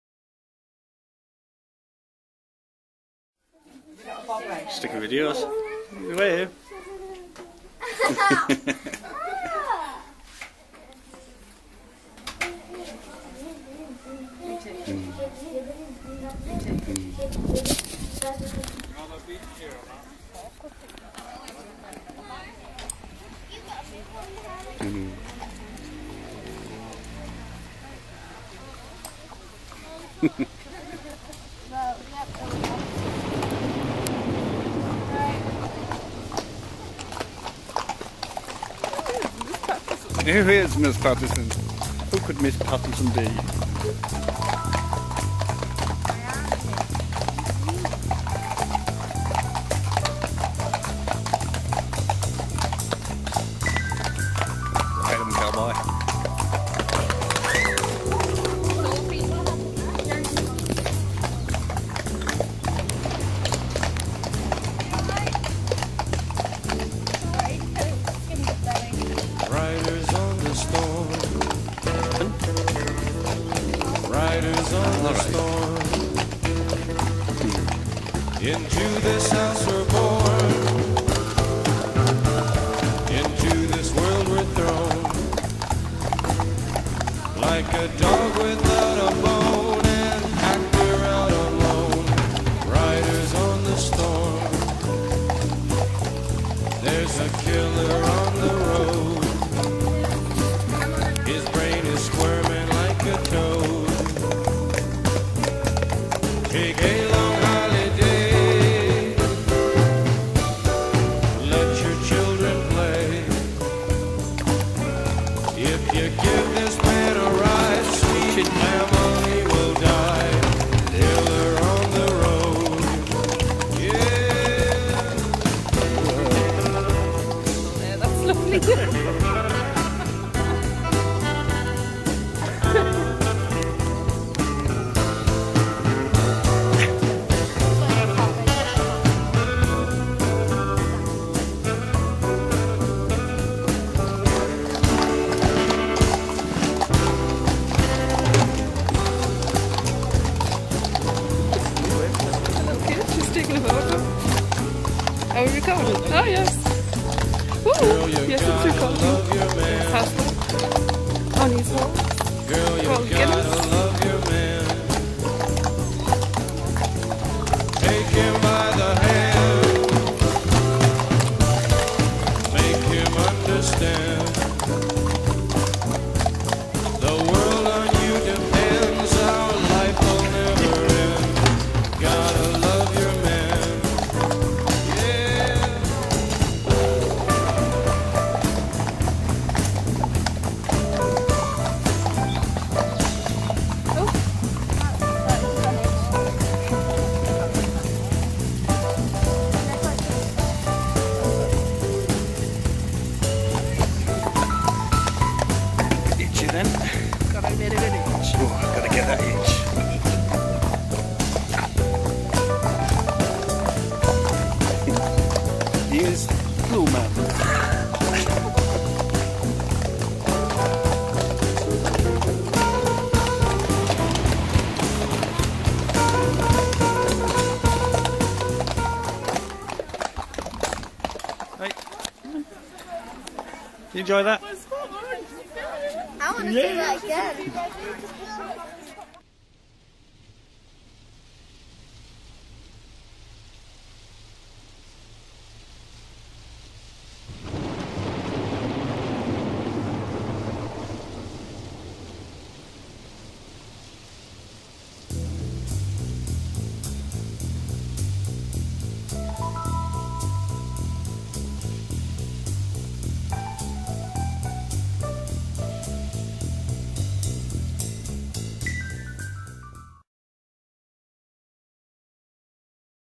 Riders in the Park...
a horse ride around Haggerston Castle...